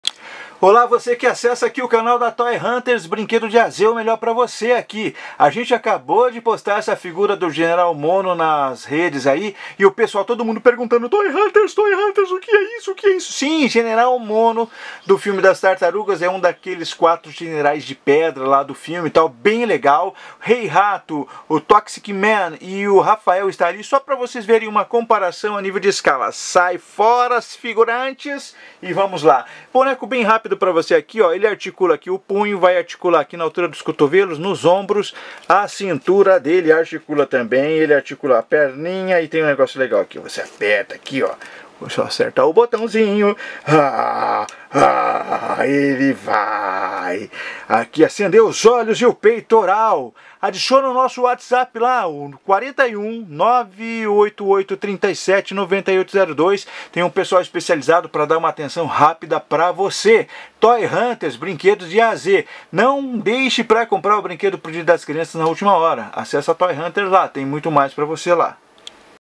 Tartarugas Ninjas General Mono Playmates Eletrônico
Boneco do personagem do General Mono da Série das Tartarugas Ninjas , Um dos Generais de  Pedra da ´serie , o boneco mede em média  17 cm  articulado e tem o sistema de luz que acende o peitoral e os olhos ,

Playmates 2006